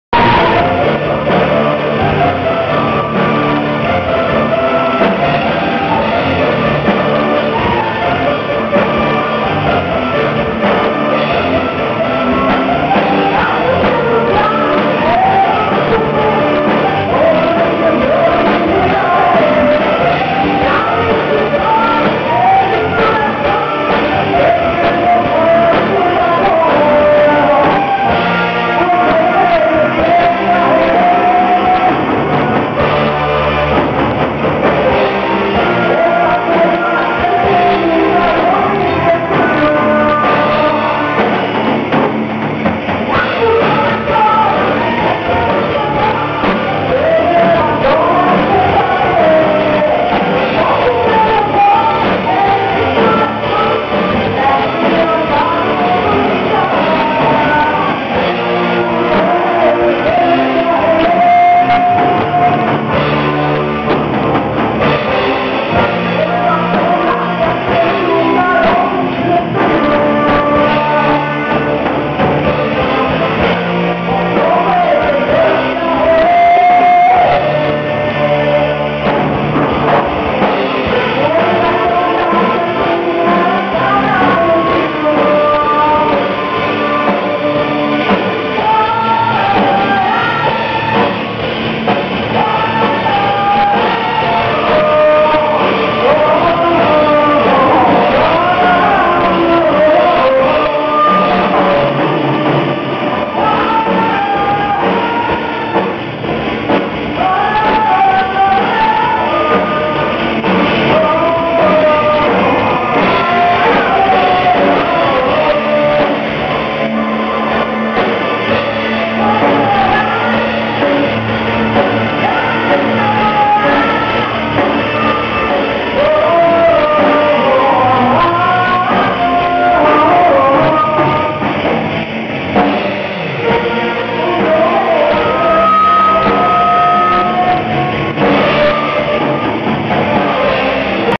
fallen angels en vivo
Fallen Angels por fin se pudo subir un video en vivo asi que bueno no es el mejor video en vivo pero seguro pondre mas videos de mi ex banda !